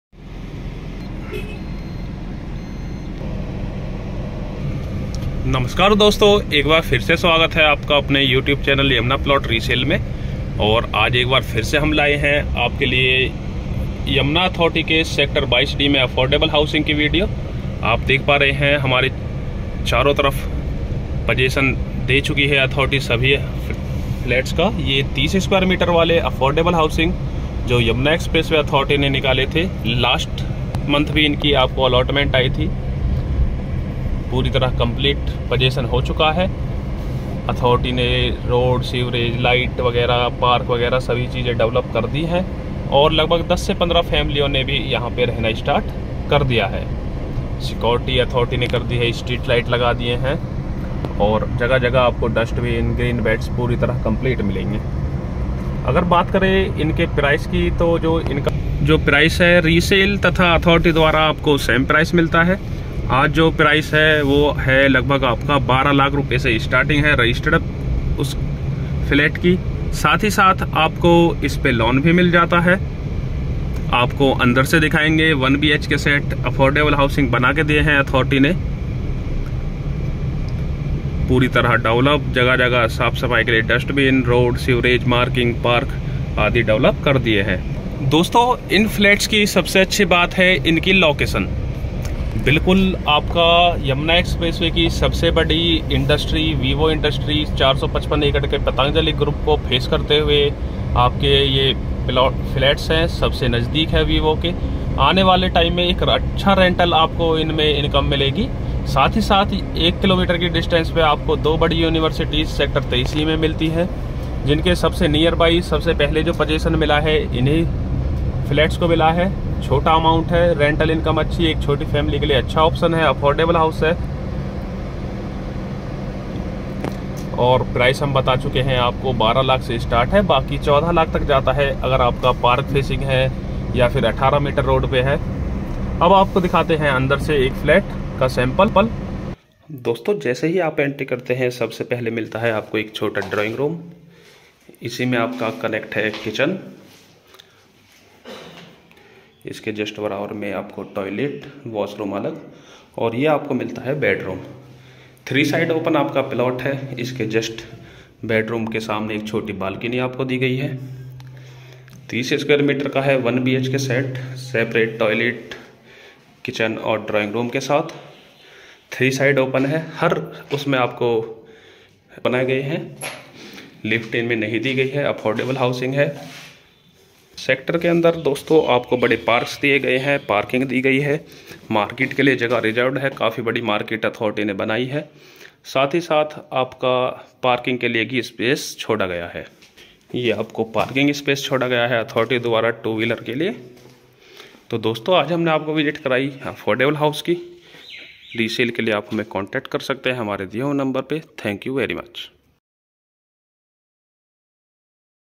30sqm Affordable House Sector-22D Yamuna Expressway Yamuna Authority
This video shoes the latest site visit , development, progress and explain a lot of information about 30sqm Affordable house Sector-22D Yamuna Authority Yamuna Expressway.These flats were allotted by Yamuna Authority in sector -22D.